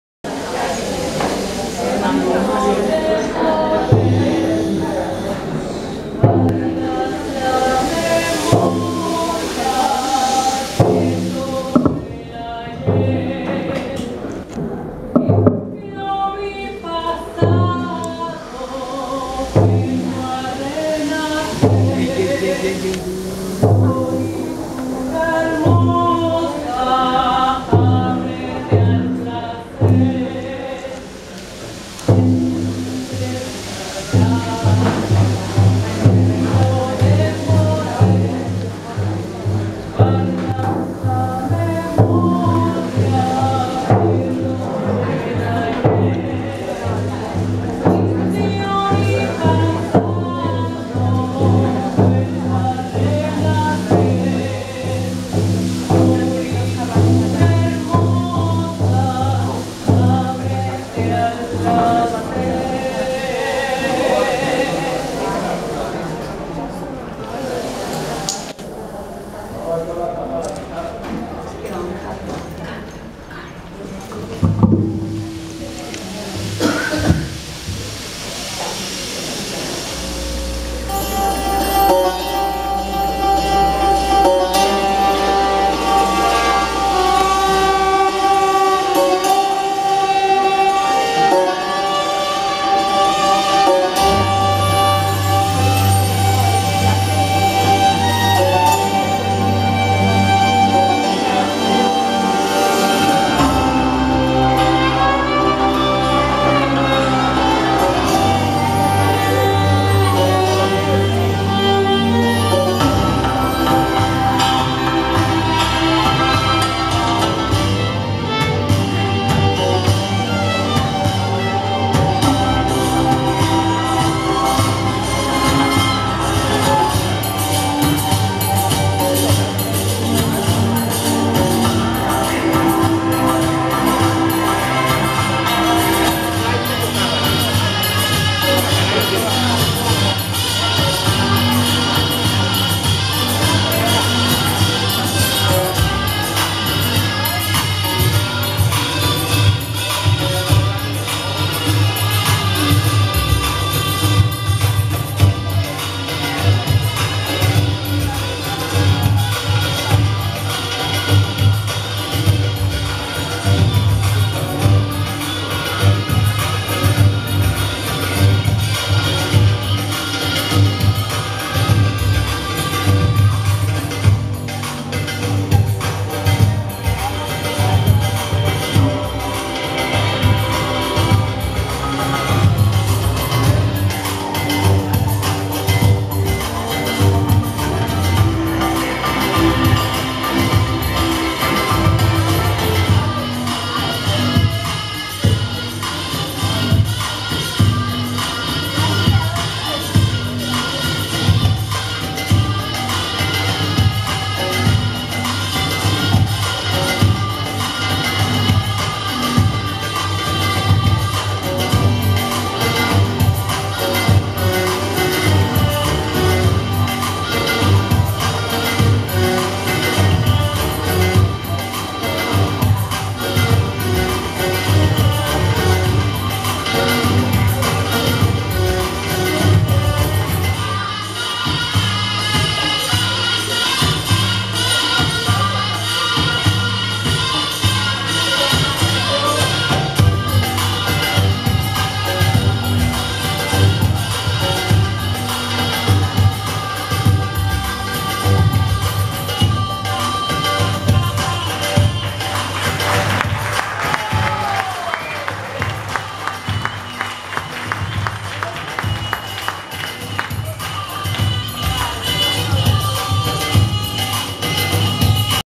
Danza Hara en el lanzamiento del
Danza Hara en el lanzamiento del Carnaval De La 13 en la Escuela Monseñor Perdomo, San Javier, Comuna 13, Medellín, Colombia.